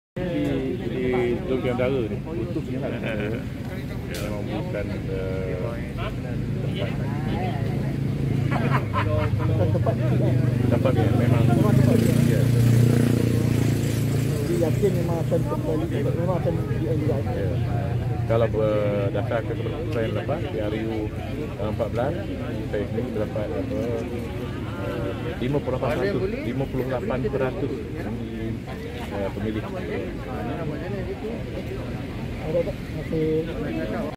"DUN Tanjung Bidara ini memang kawasan BN" - Datuk Md Rawi
8 Nov - Bekas ADUN Tanjung Bidara, Datuk Wira Md Rawi Mahmud menjangkakan kemenangan sekali lagi akan menjadi milik calon Barisan Nasional (BN), Datuk Seri Ab Rauf Yusoh dalam Pilihan Raya Negeri (PRN), 20 November ini.

Ia memandangkan sokongan penduduk di kawasan itu yang begitu kuat kepada BN pada PRU-14.